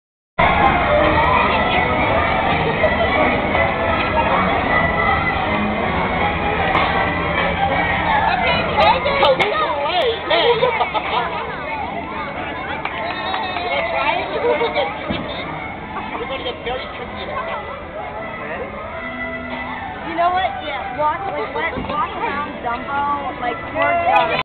Hatter Alice Hopscotch 2
Hatter and Alice play hopscotch with their young guests during Magical Beginnings.